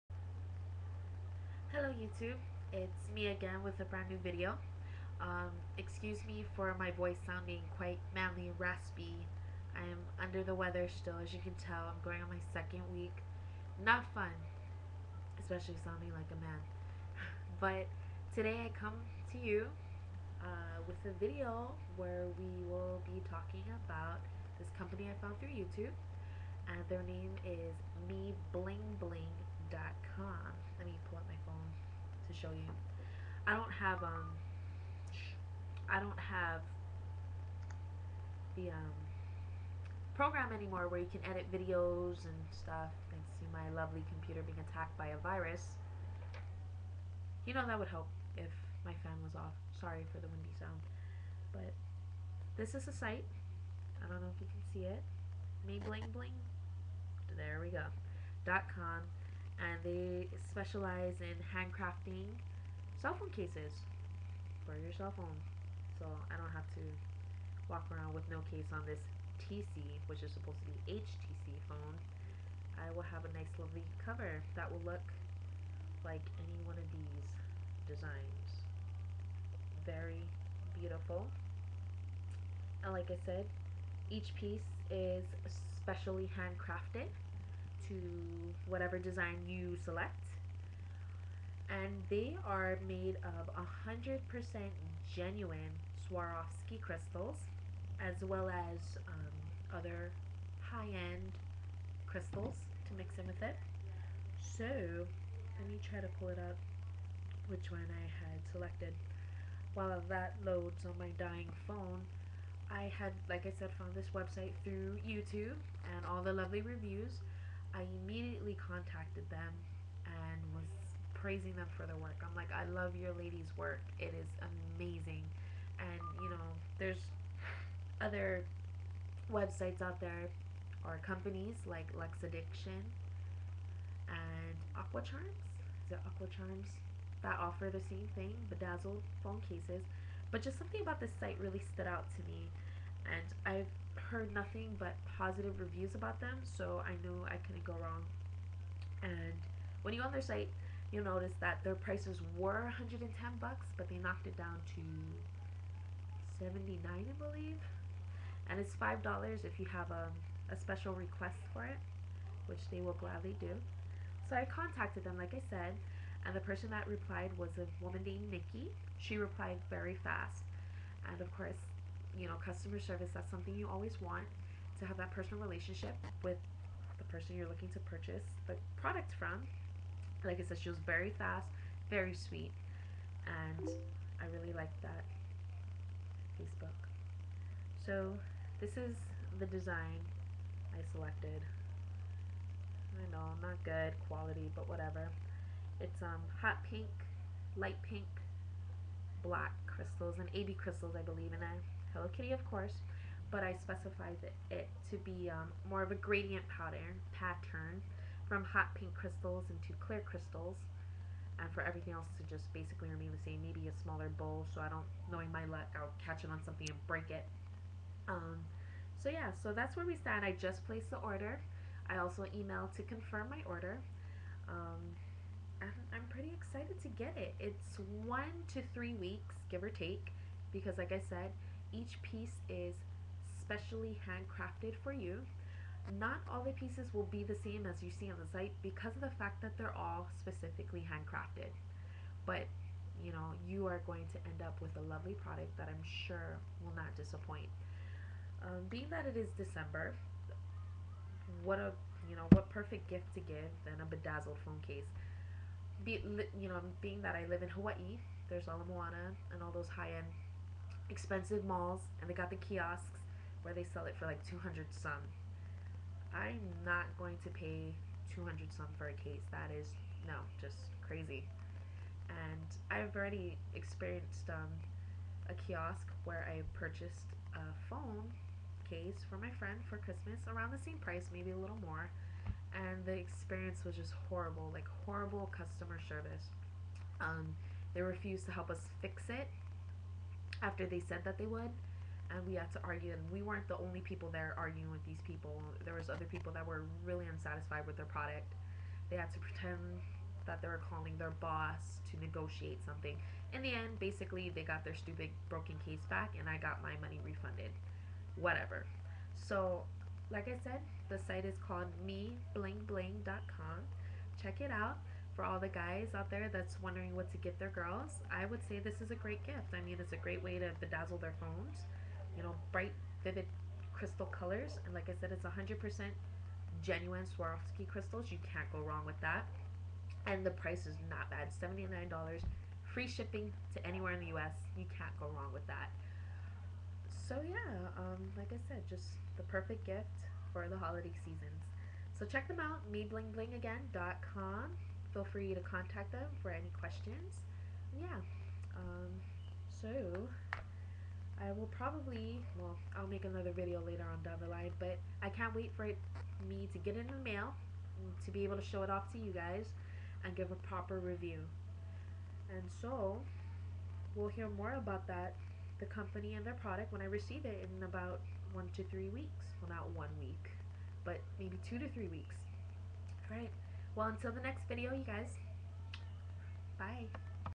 MeBlingBling.com Pre-Review
I just placed my order to the for the bedazzled phone case I've been wanting! An early Christmas gift to myself from myself! O__o Decided to make this "pre-review" video right after I got done placing my order, to let more people know about this site and I was extremely happy and satisfied with it, why not share it with everyone else?! :) For a bedazzled phone case, I'm telling you a GREAT price and truly a beautiful, lovely, attractive holiday gift!